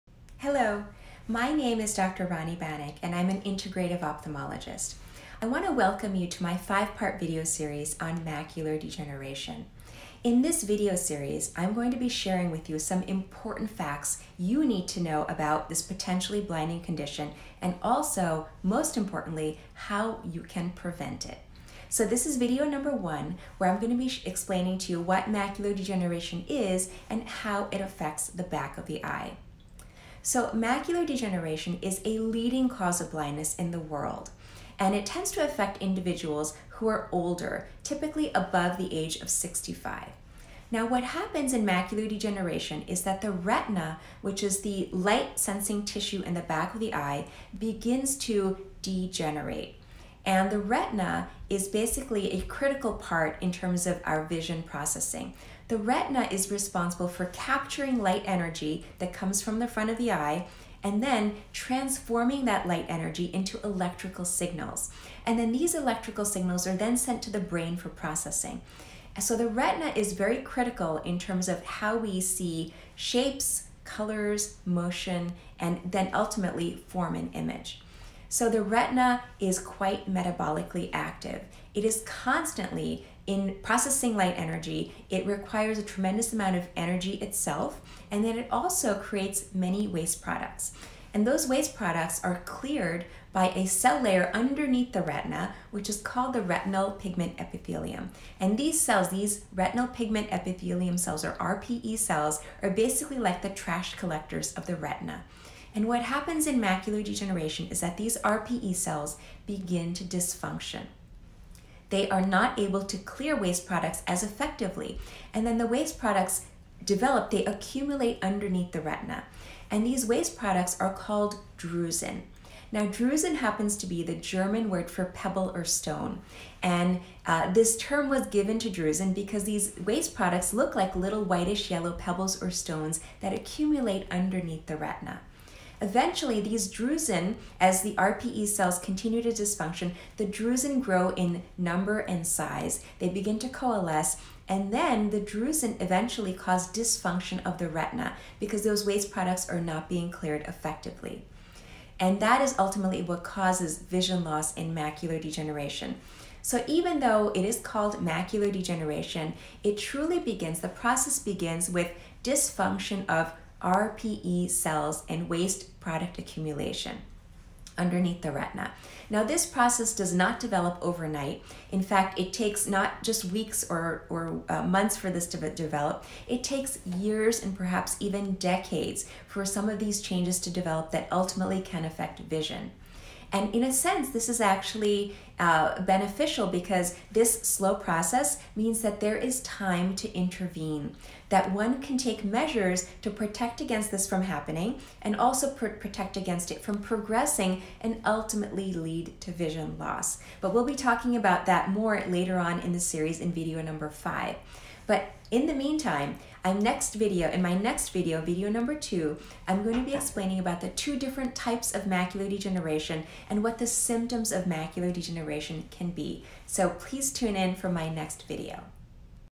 What is Macular Degeneration?
In this 5-part video series, board-certified integrative ophthalmologist, Dr. Rani Banik, shares important information about Macular Degeneration, a leading cause of vision loss and blindness in the world. In Video #1, she explains what Macular Degeneration is and how it affects the back of the eye.

Dr. Rani's passion is to help people prevent vision loss from this potentially devastating eye disease using evidence- based nutrition and lifestyle interventions.